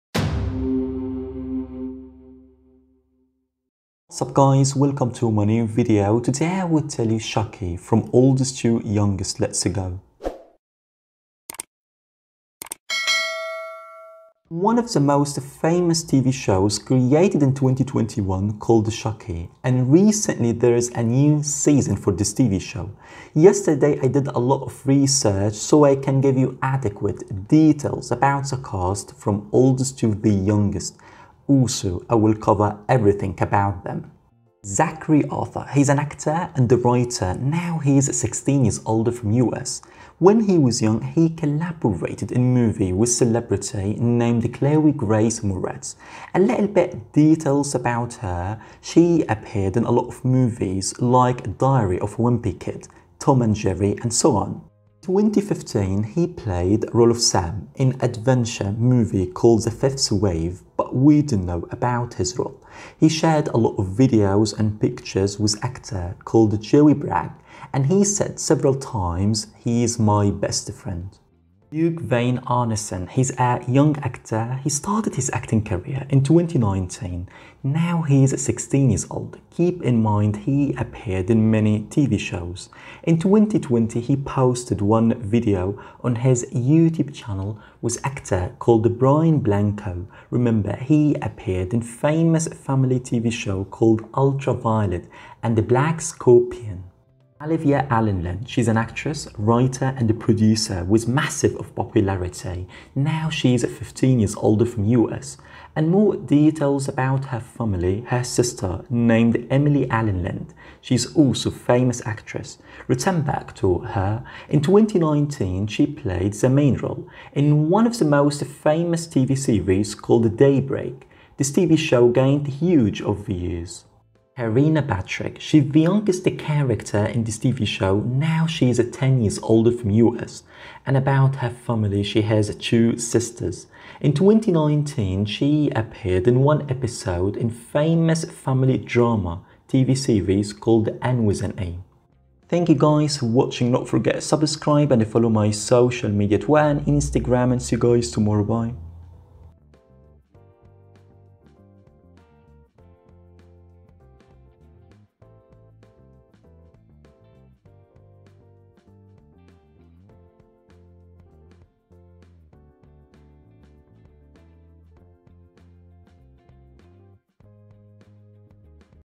Chucky (Season 2) From Oldest To Youngest
Chucky Season 2
Today I'll talk about TV series called "Chucky" also I'll let you know information about the cast from oldest to youngest.
The full cast:
Zackary Arthur
Bjorgvin Arnarson
Alyvia Alyn Lind
Brad Dourif
Barbara Alyn Woods